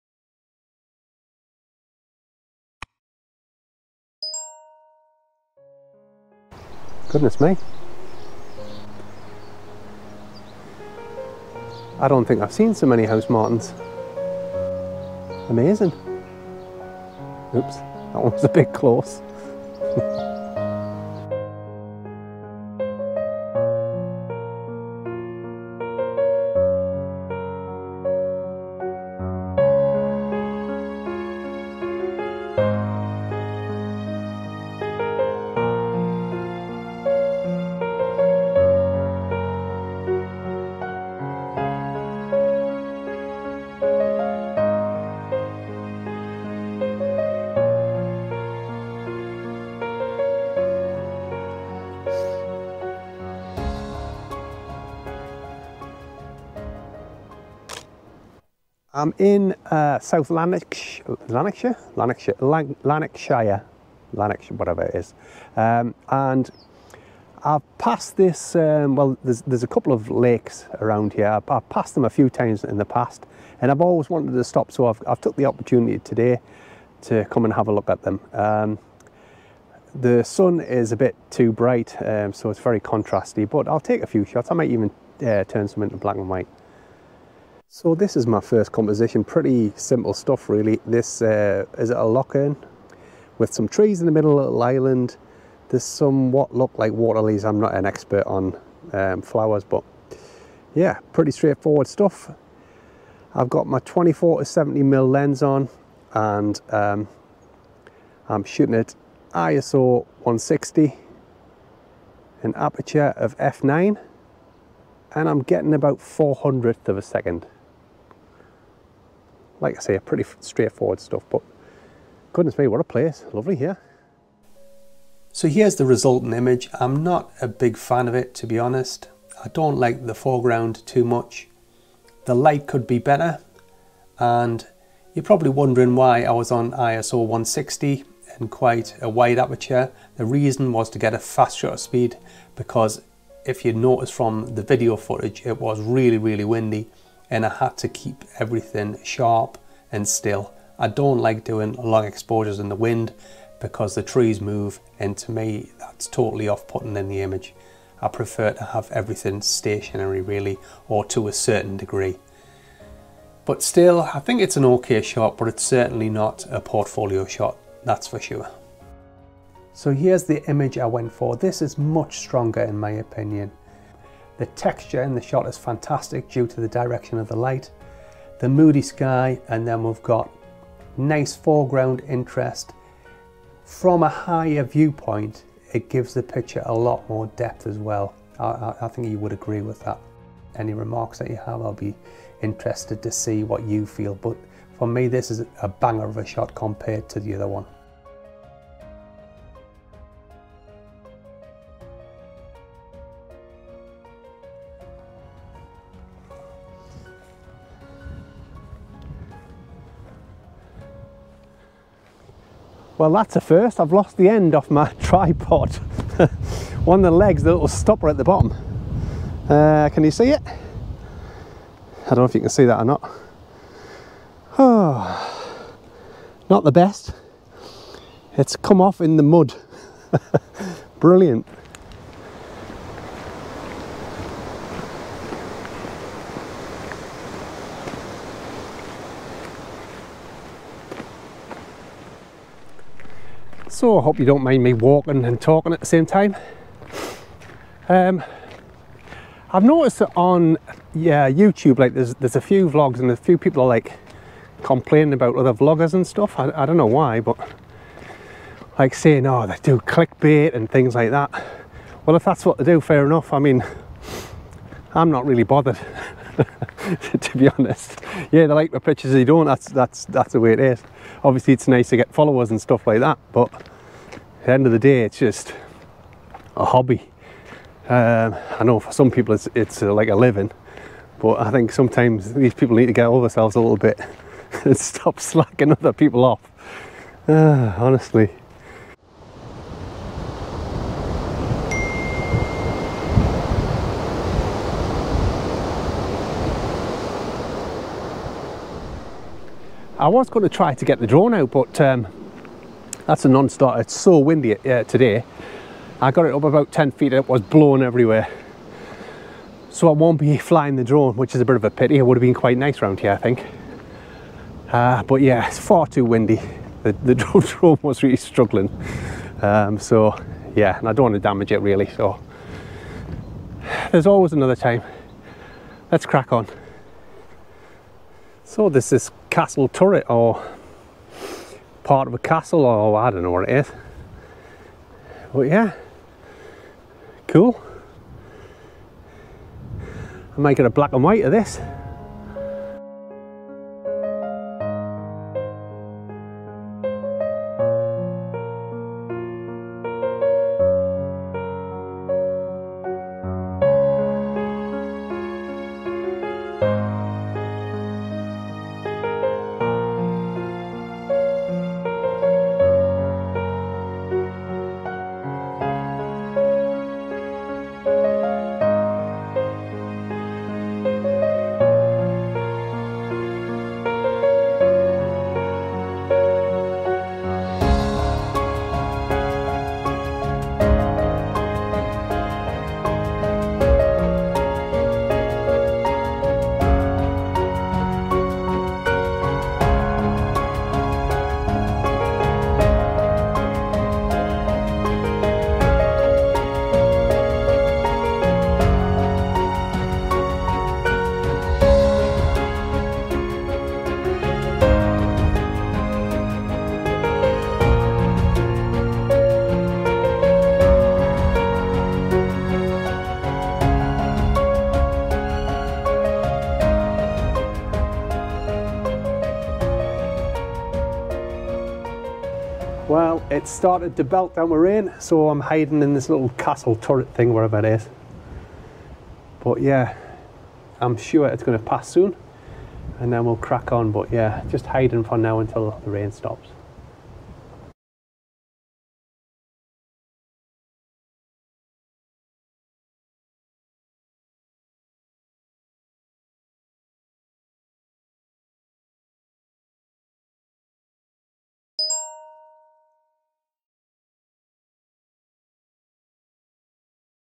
WINDY DAYS - Landscape Photography
Join me on another photo journey through the wild and rugged landscapes of Scotland, where nature's forces put my vlogging skills to the test. In this captivating vlog, witness the relentless high winds and torrential rain showers that challenged me to think creatively, pushing the boundaries of my videography. Although my plans to fly the drone were thwarted, the weather couldn't dampen my spirits!

Don't let the unfavorable weather fool you; this vlog is a testament to the undying passion and dedication that drives every vlogger's spirit. So grab your raincoat, buckle up, and get ready to embark on a virtual adventure. Join me as we navigate through Scotland's relentless weather, uncovering its hidden gems and capturing moments that will leave you spellbound.